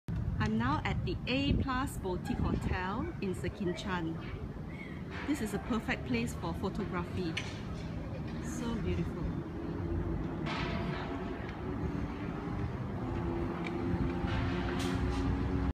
A Plus Boutique Hotel, Sekinchan
Be amazed when you step into the garden rooftop of A+ Boutique Hotel in Sekinchan. So Photography Worthy!